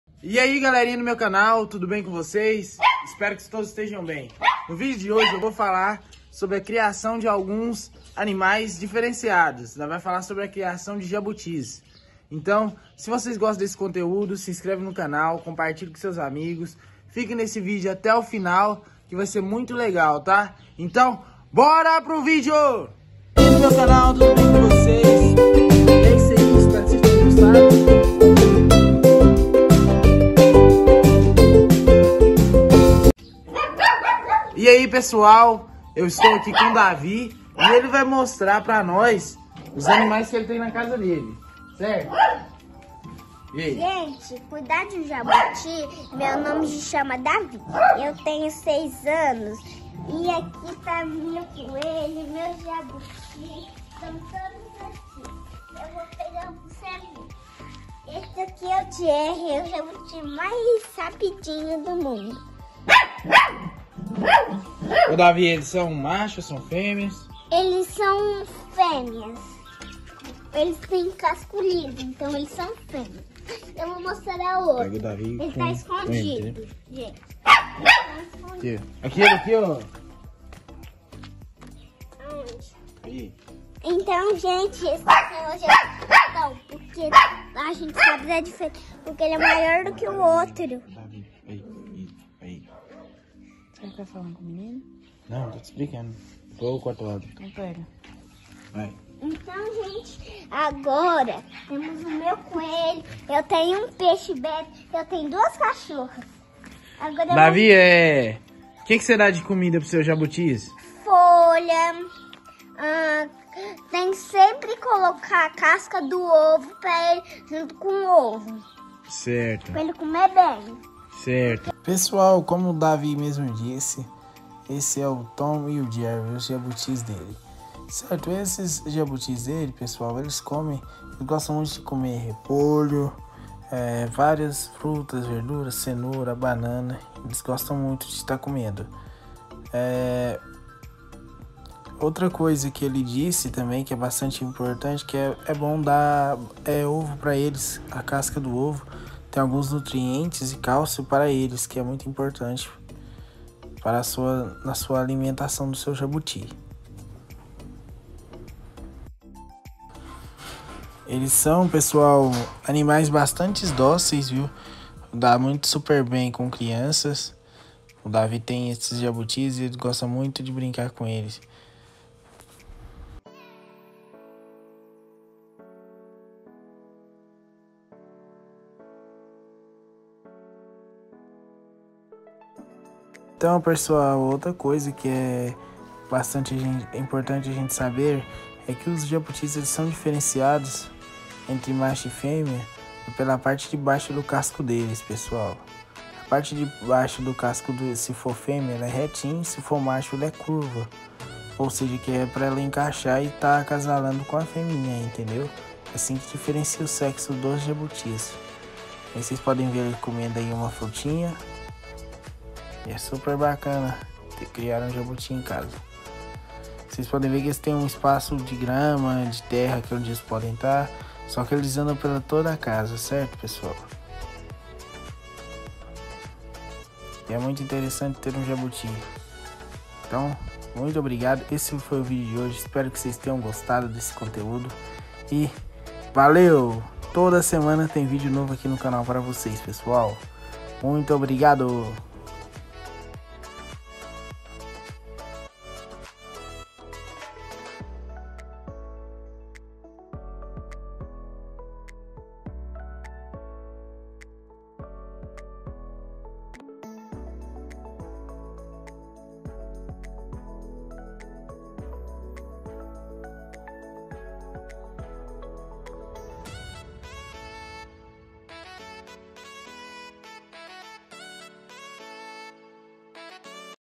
COMO CUIDAR DE UM JABUTI DE ESTIMAÇÃO!!!
No vídeo de hoje mostro um pouco de como é ter um jabuti de estimação , você que está pensando em ter um bichinho desse em casa assista esse vídeo aí e deixem o Like !!

Obrigado !!!